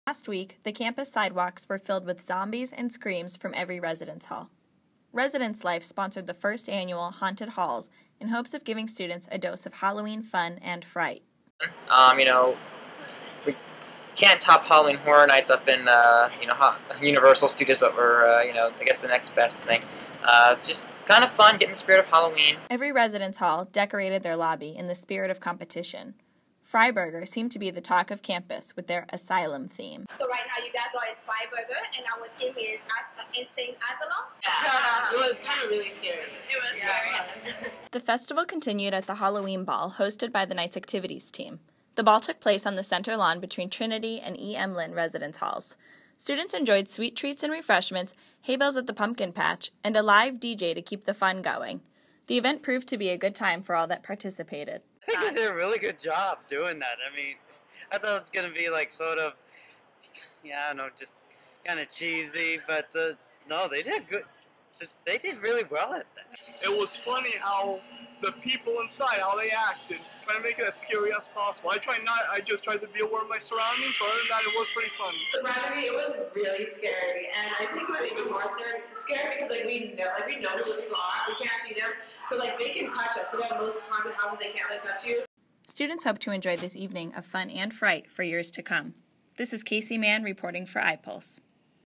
Lynn University's Haunted Halls 2013
Residence Life and the Knight's Activities Team hosted the first annual Haunted Halls on campus in October.  Each dormitory lobby was decorated to reflect a certain theme that represented a haunted house at Lynn. Students enjoyed the event and look forward to the event for years to come.